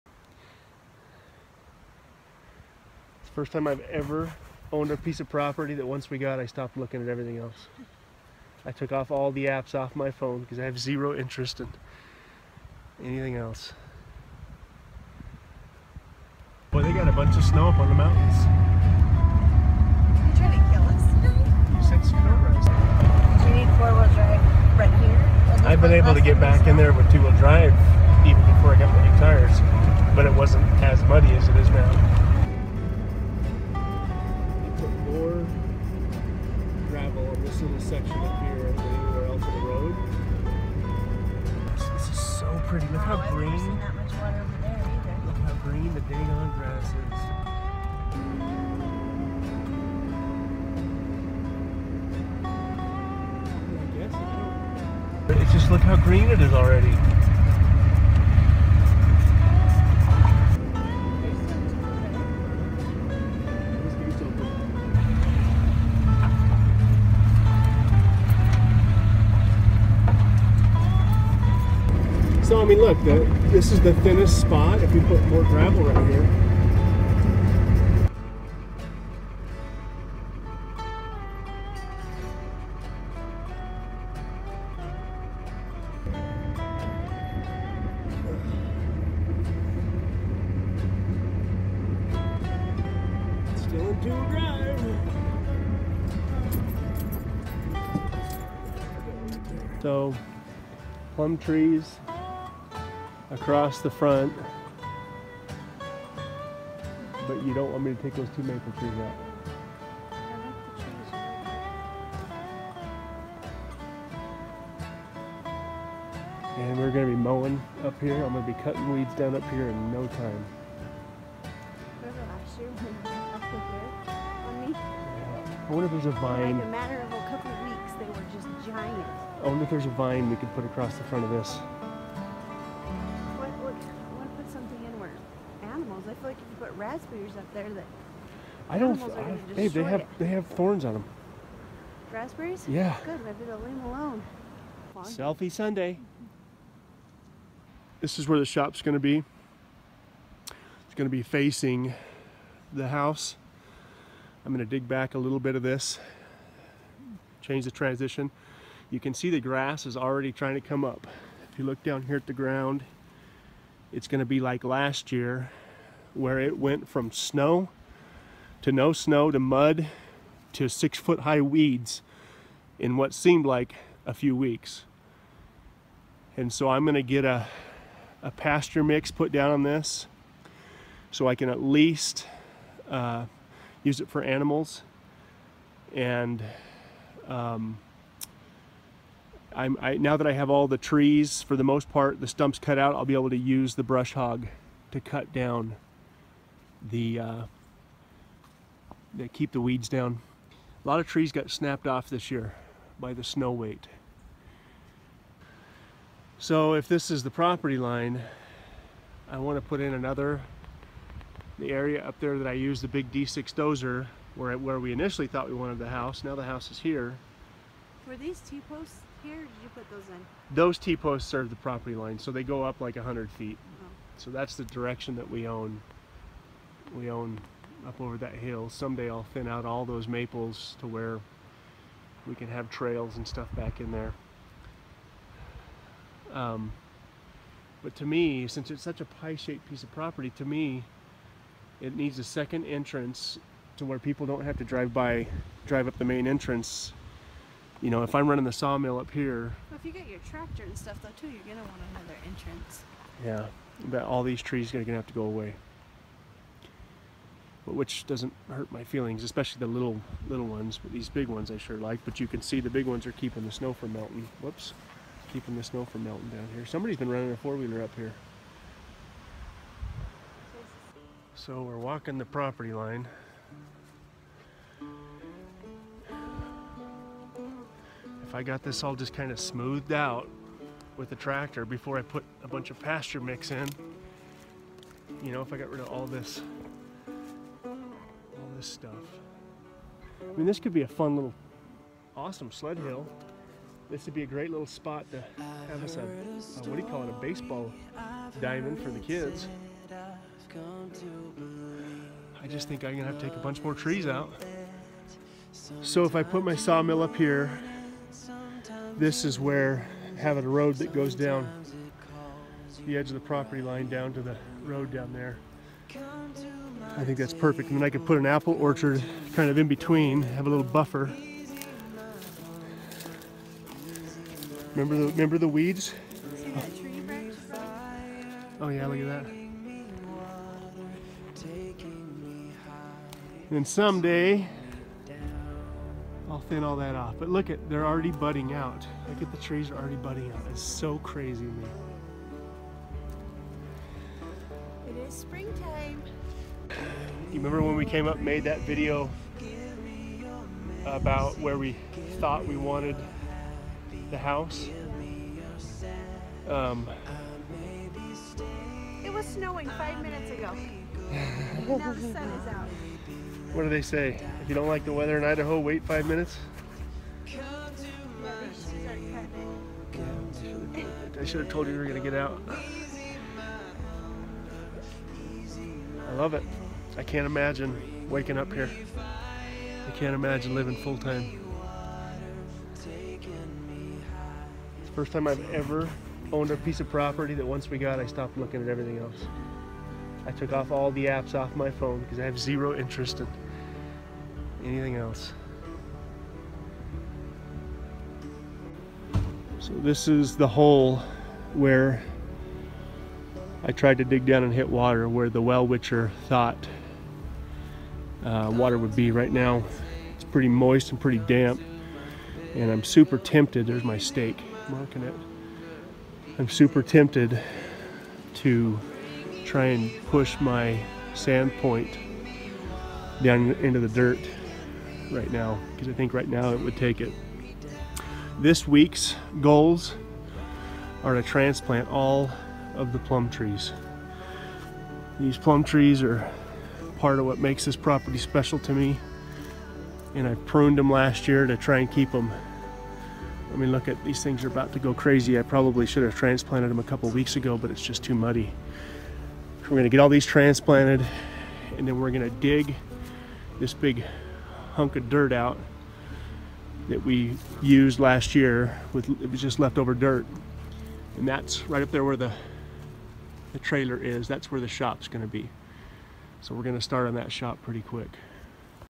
#79 - Cedar (Wifey) Thinks I'm cutting Down Too Many Trees...
We wanted to see how much snow was left and if it had dried out although it's been raining today. We jumped in the truck and ran up to the ranch. No more snow... Can't wait to dive back in!